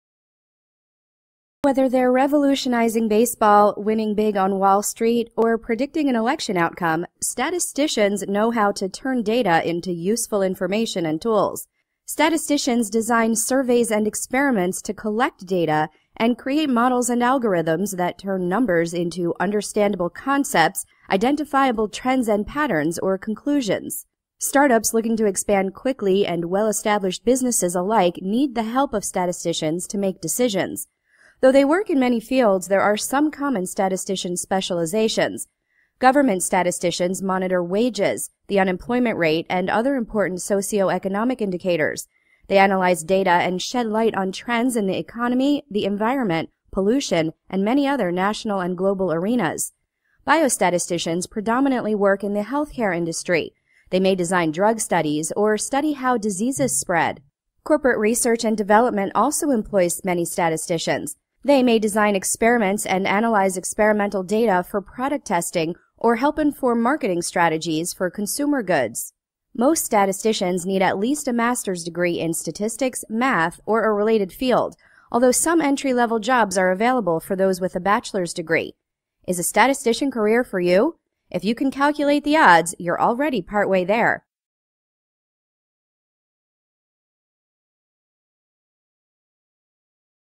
15-2041.00 - Statisticians
Develop or apply mathematical or statistical theory and methods to collect, organize, interpret, and summarize numerical data to provide usable information. May specialize in fields such as bio-statistics, agricultural statistics, business statistics, or economic statistics. Includes mathematical and survey statisticians.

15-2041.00